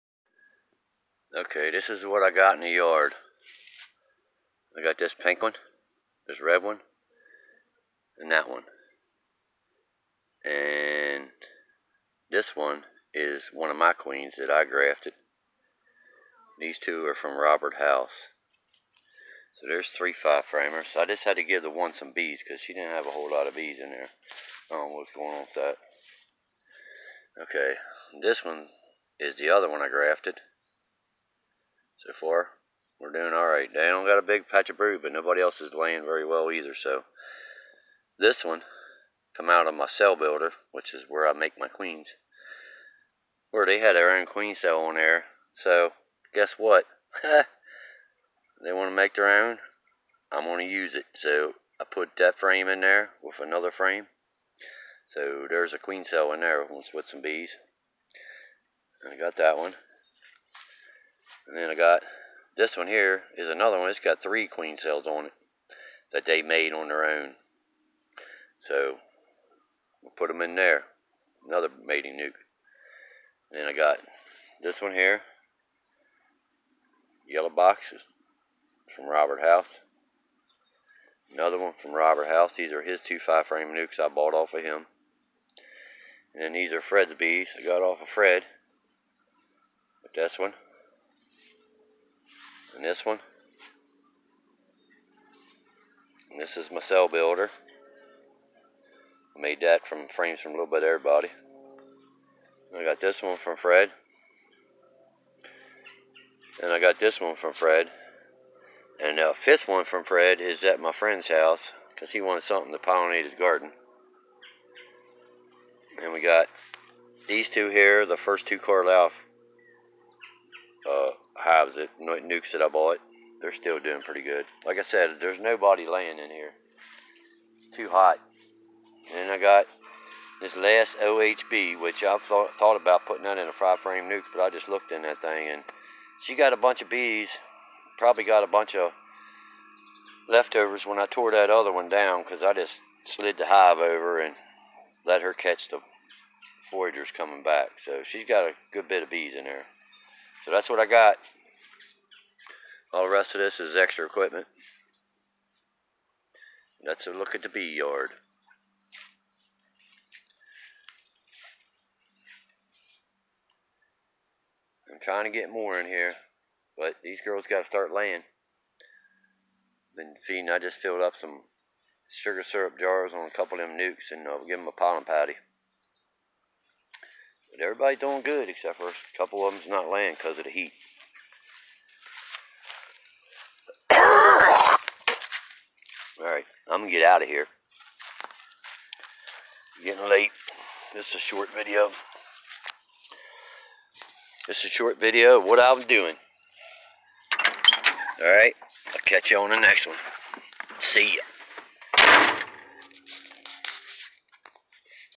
The yard and made 2 nucs.
This is a short run down of my yard. I just made 2 two frame nucs with cells on the frames of my cell builder. This is why you add all the brood at one time. I came back after I installed my graft and switched from a 5 to a 10 frame box. So there was probably larvae the bees could make queens from and thats exactly what they did. Only problem for me is they used plastic foundation to pick from and its almost impossible to cut the off without destroying them. So you take the whole frame. And the first one out is the chosen one. I also lifted the lid on the last Ohb queen and she has about 4 frames to 5 of bees. Not sure since i did pull frames how many nurse bees but im guessing from the last time i was in there they are mostly foragers and won't be there long. Also none of my queens are laying. Its been to hot here. In the high 90s which probably makes it over 110 in the box. So im looking to make some screened inner covers and vent shims.